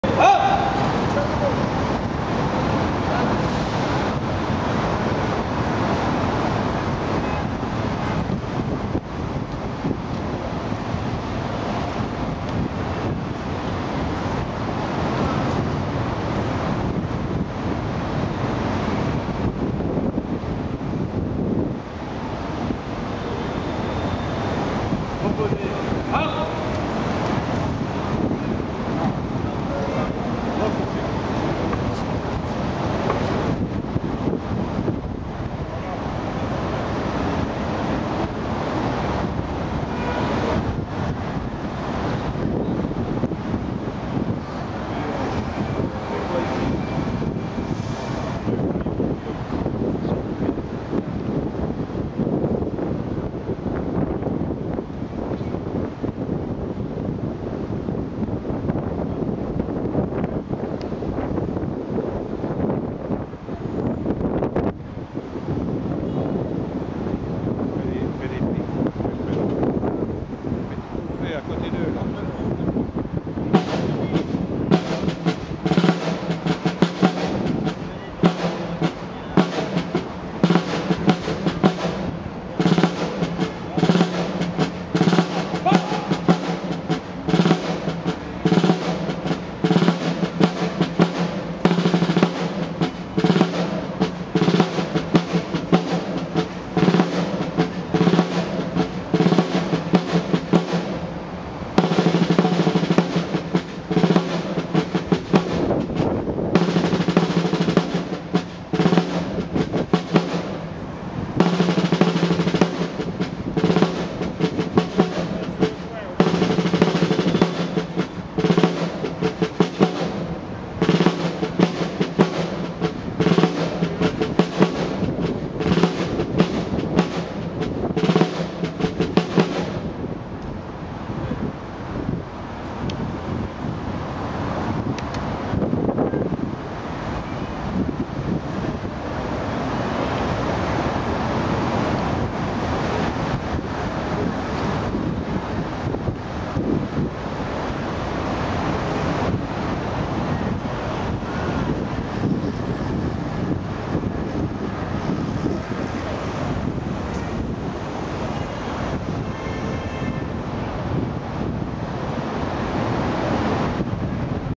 Tomb of the Unknown Soldier, video 1
The dedication of the eternal flame at the tomb of the unknown soldier, 26th August 2011